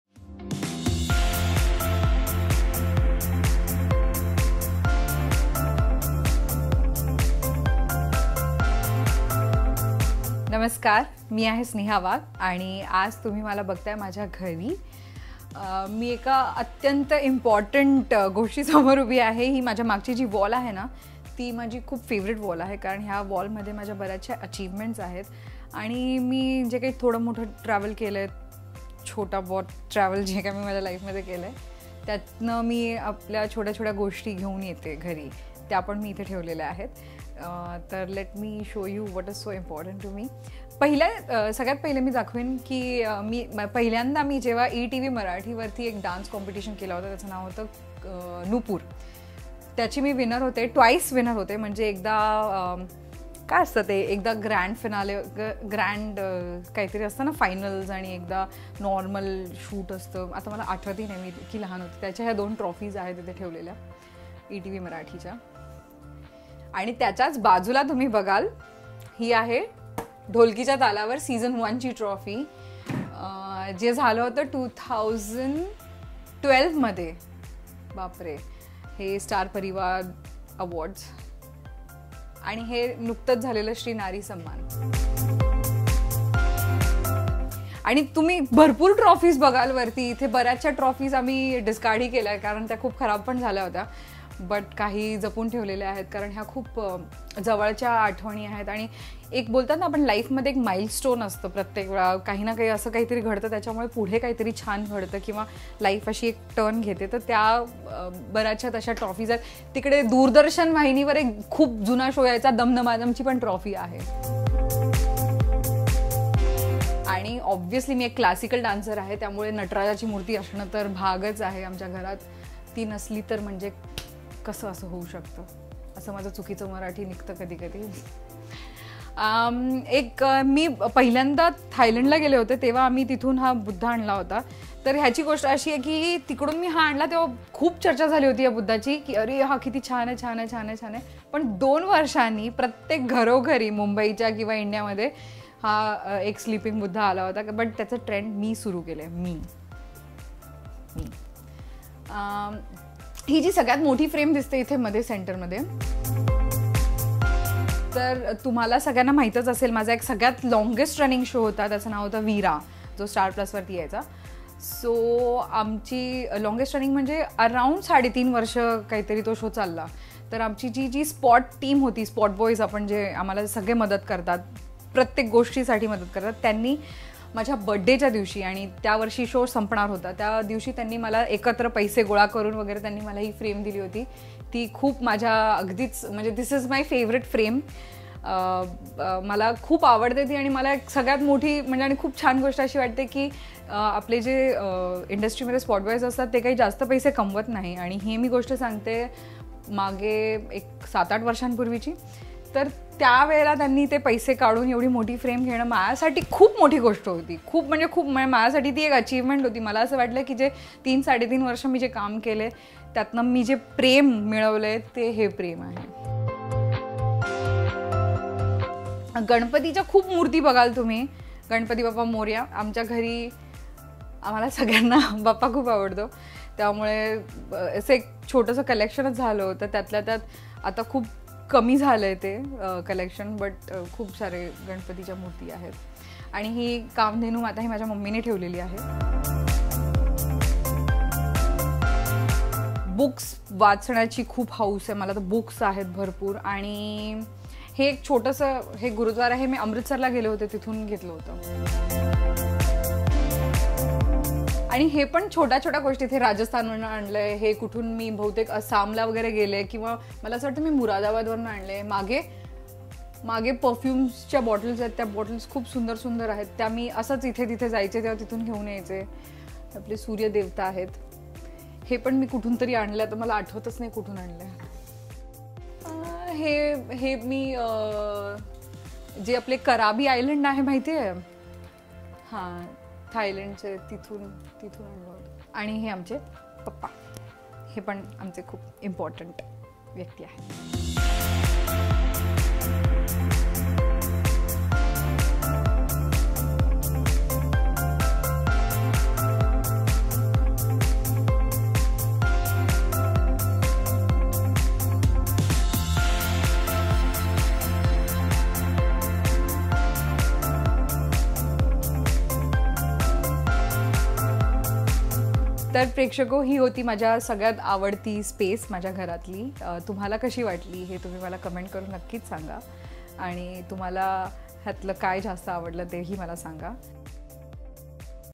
EXCLUSIVE : स्नेहा वाघच्या घराची सफर | Sneha Wagh | Bigg Boss Marathi S3
अभिनेत्री स्नेहा वाघ  बिग बॉस मराठी 3च्या घरात आहे. आज आम्ही तुम्हाला स्नेहा वाघचं घर दाखवणार आहोत. पहा तिच्या घराची झलक या व्हिडीओमध्ये.